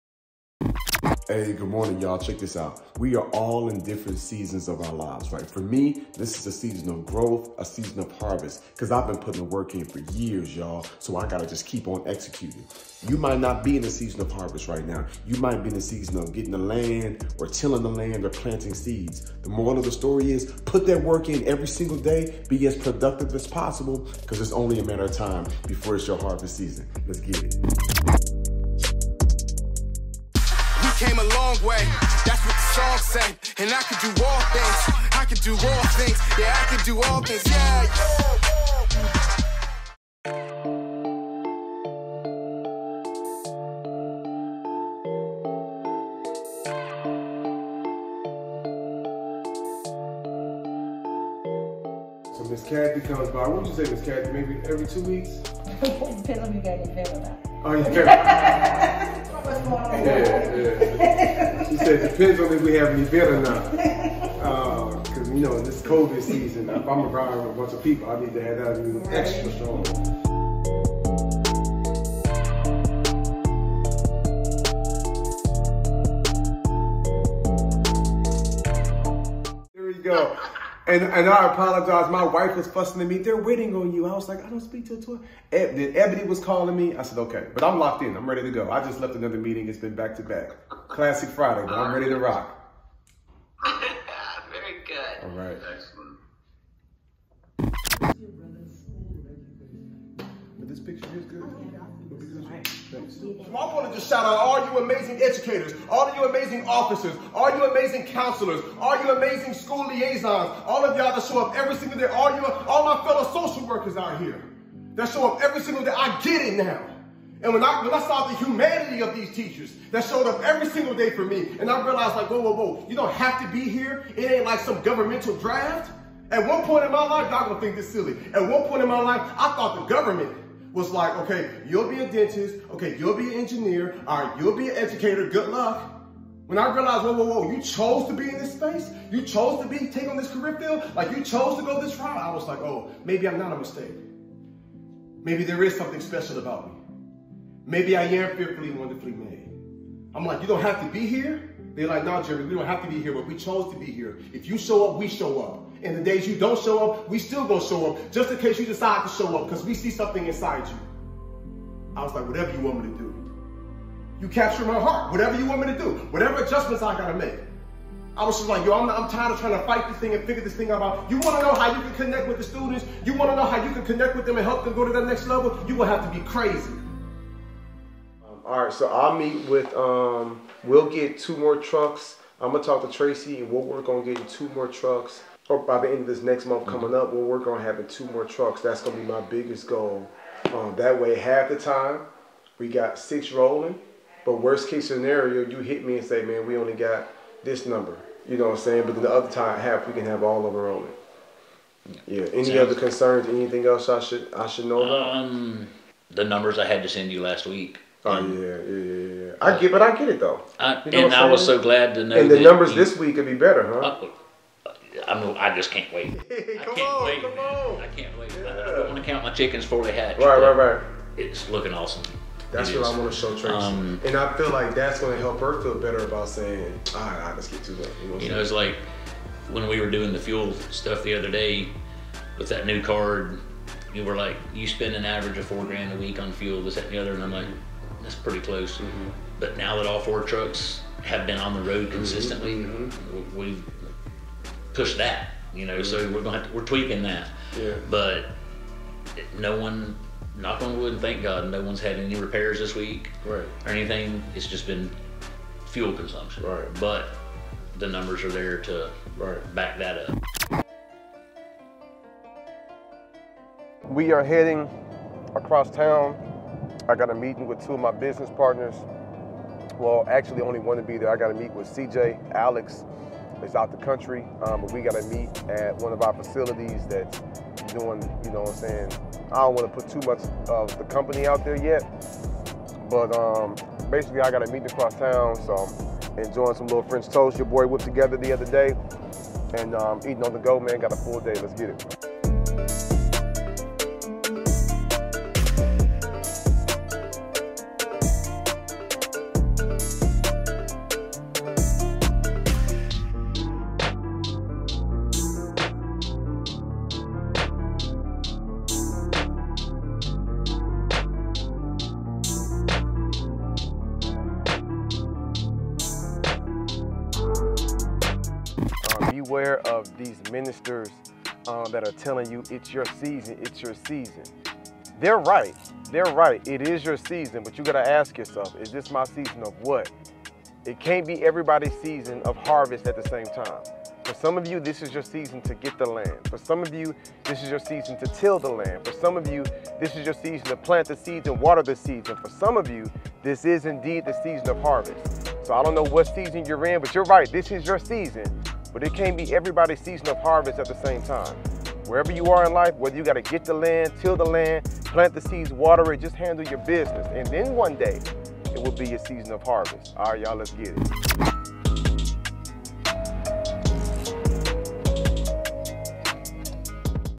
A Day In The Life As A Motivational Speaker w/ Jeremy Anderson Ep. 5 " The Season Of Harvest "
Do you desire to become a paid professional or motivational speaker?

“It can’t be everybody’s season of harvest at the same time.”

Trust the process. A plant doesn’t grow to bear fruit overnight. One must first plan during the winter months to choose your plot. Not until the next season can you till the soil and plant your seeds. Next, you nurture and water before you can reap the harvest.

No matter what season you’re in, Jeremy encourages you to commit and execute to your highest potential.